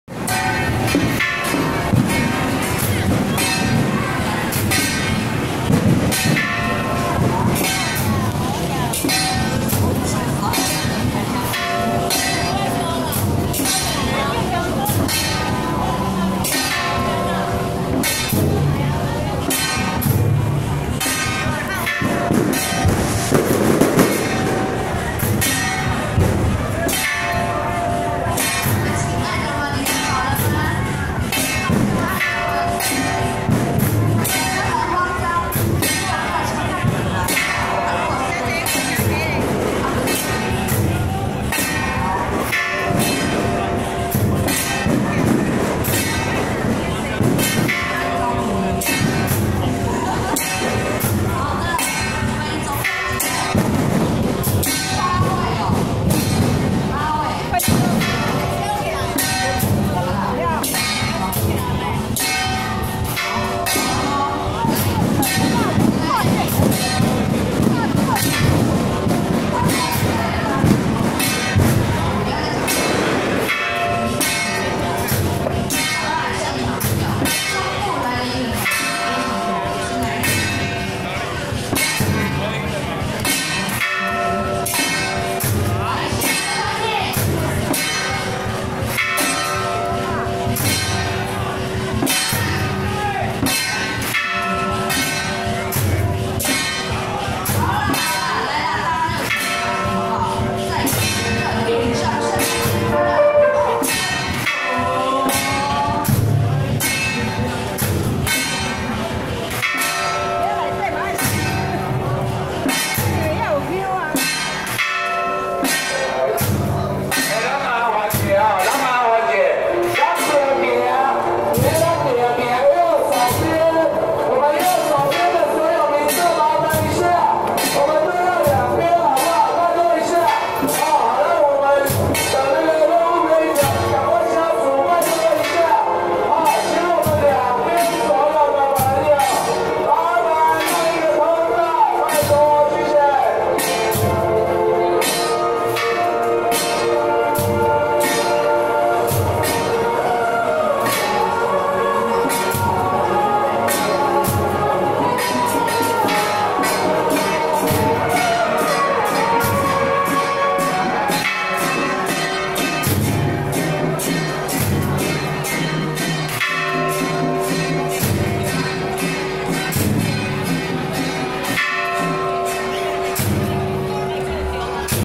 107桃園菓林太子宮恭迎中壇元帥巡境賜福遶境【台南新營-十二婆姐陣】
台南新營-十二婆姐陣